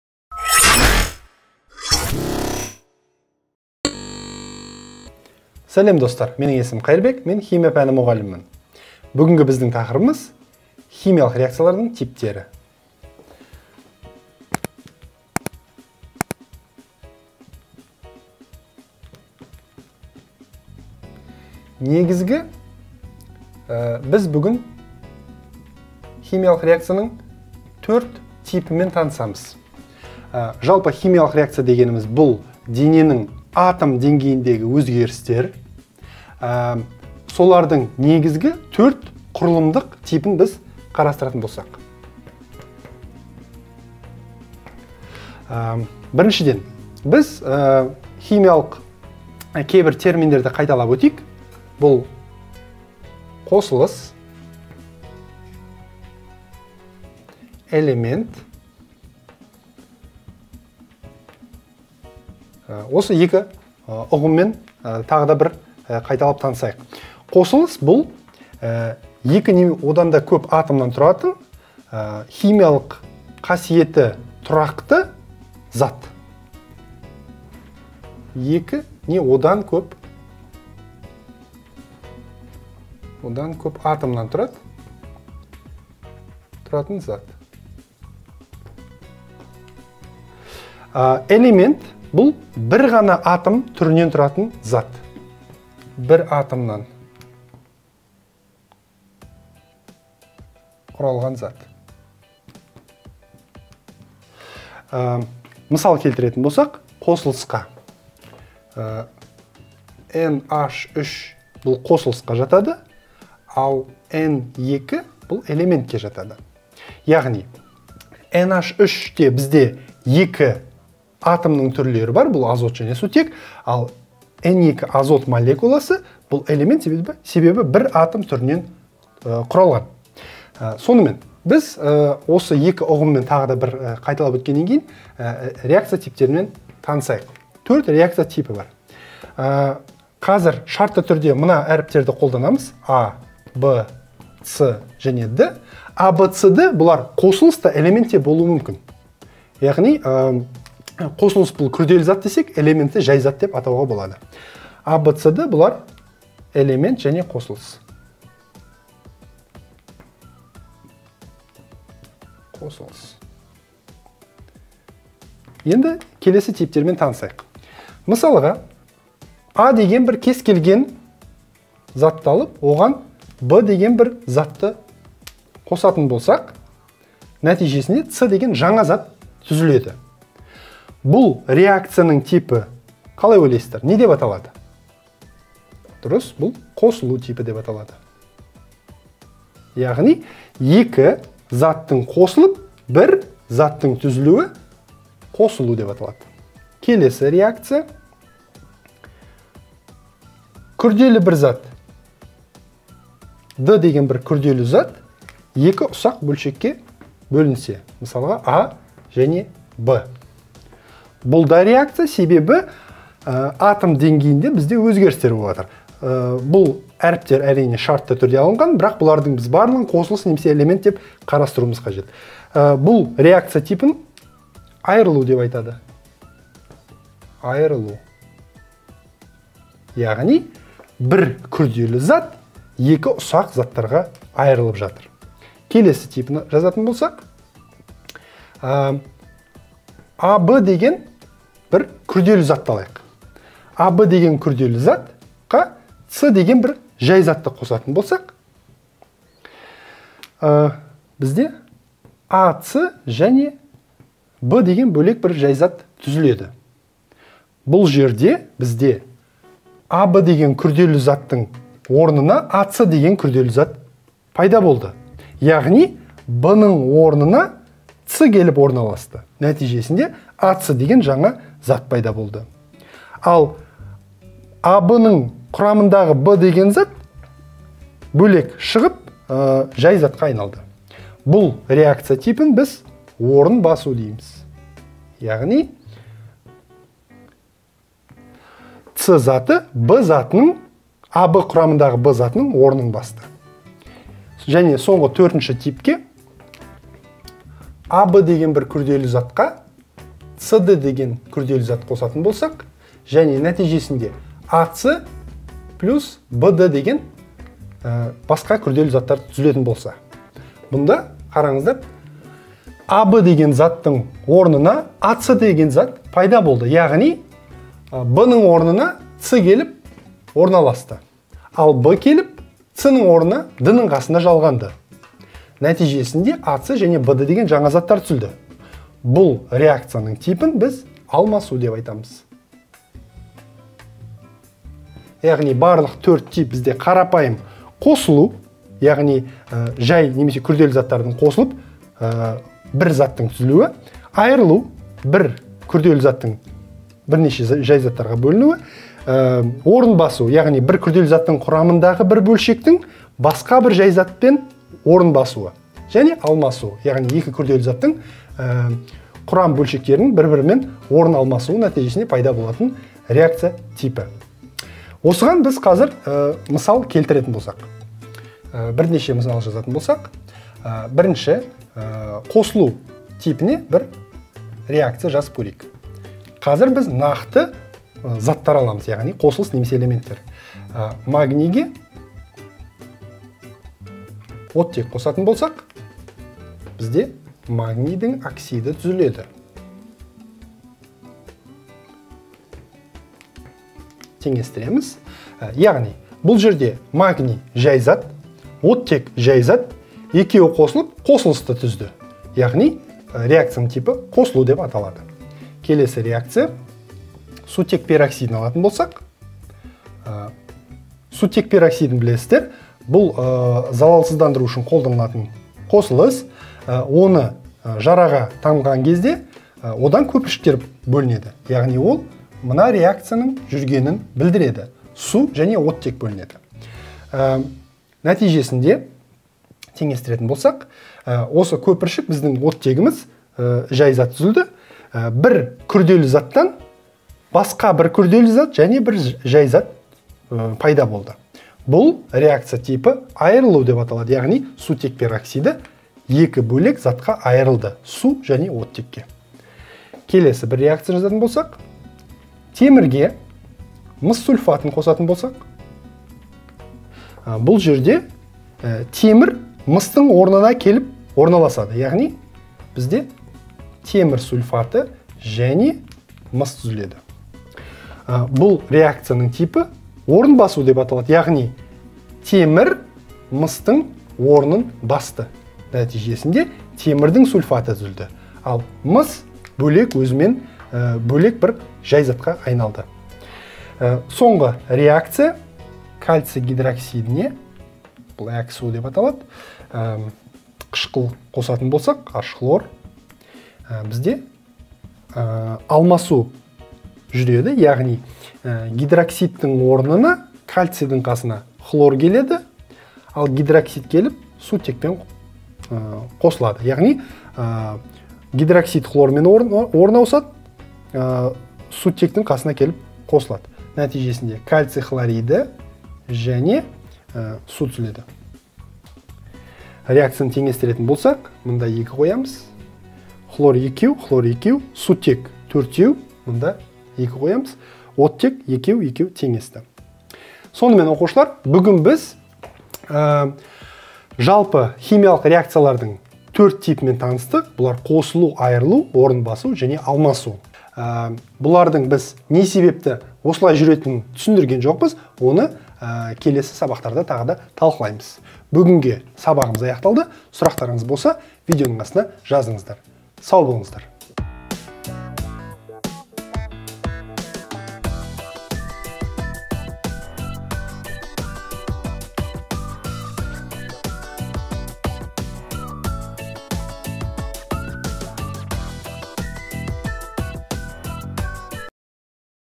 Химиялық реакциялардың типтері. 8 сынып.
MEKTEП OnLine - Білім беру жобасы!

Кайырбек Буксукбаев - «Назарбаев Зияткерлік Мектептері» мұғалімі.